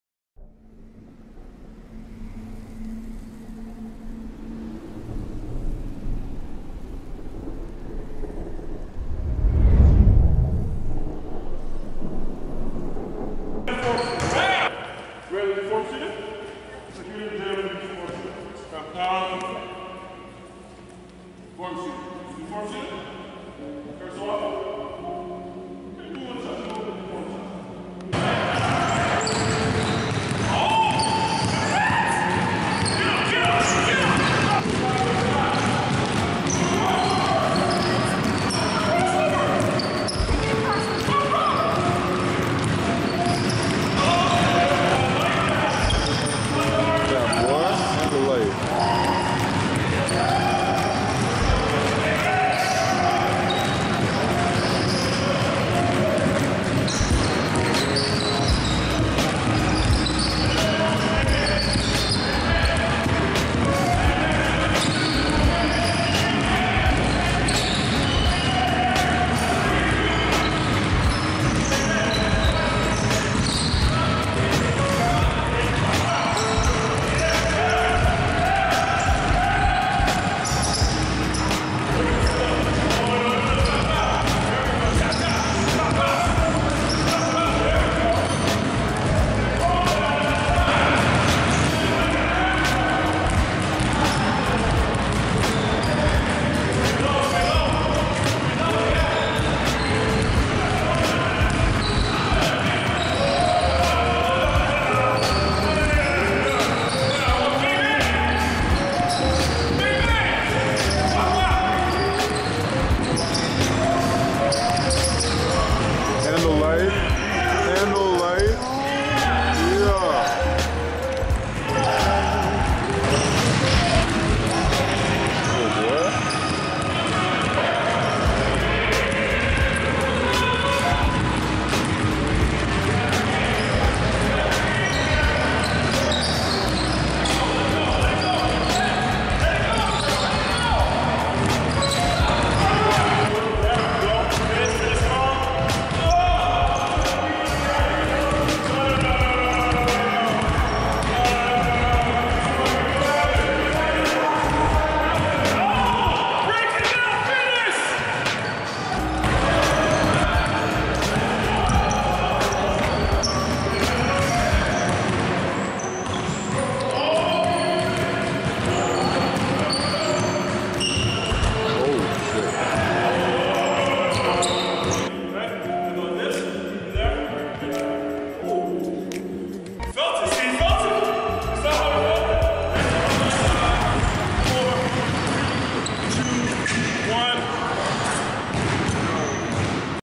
Athlete Institute | Educated Baller - Session 1
Youth Spring Development basketball training at the athlete institute.